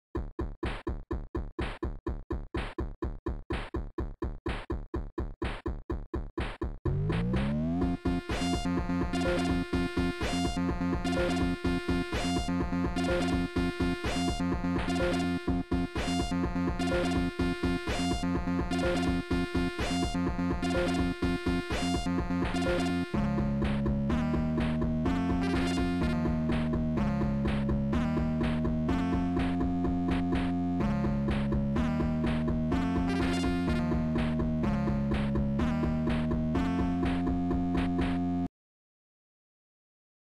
Milkytracker....
I'm bad at using it...

I'll keep practicing though, but I wish there was good tutorial on tracker composition.